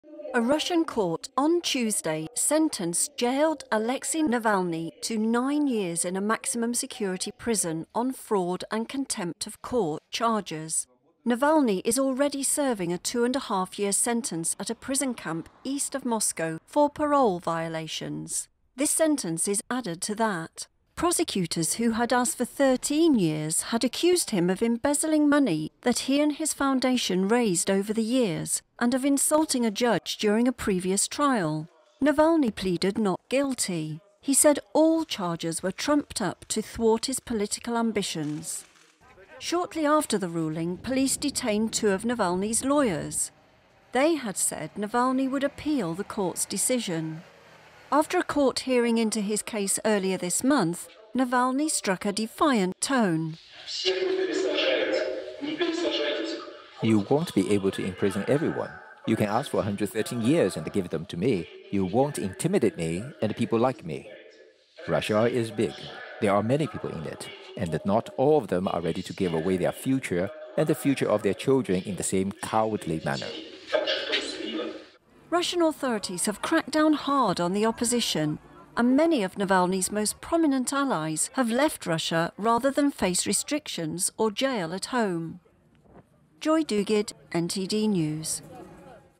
Court Sentences Navalny to 9 More Years | NTD UK News
NTD UK News: 22 March 2022

Court Sentences Navalny to 9 More Years

Russian Kremlin critic Alexei Navalny has been sentenced to nine more years. A judge convicted him of fraud and contempt of court. Critics see the trial as an attempt to keep President Vladimir Putin's most ardent foe in prison for as long as possible.

Full Show Broadcast at 21:00 GMT/BST Every Weekday
On Sky Channel 190, or Select NTD on Channelbox: Freeview Channel 271